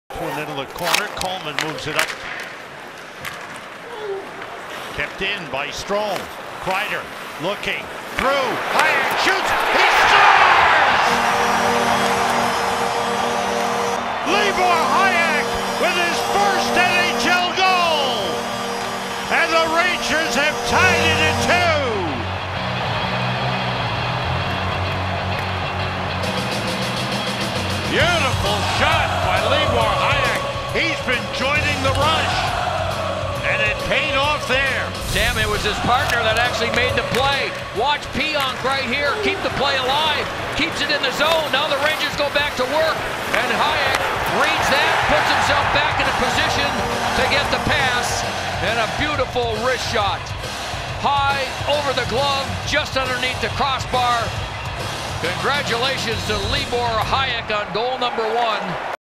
Libor Hajek #43 (New York Rangers) first NHL goal Mar 9, 2019
Assist: Chris Kreider, Ryan Strome.
Goalie: Cory Schneider.

"Libor Hajek
Born: February 4, 1998
Birthplace: Smrcek, CZE
Shoots: Left
Draft: 2016 TBL, 2nd rd, 7th pk (37th overall)"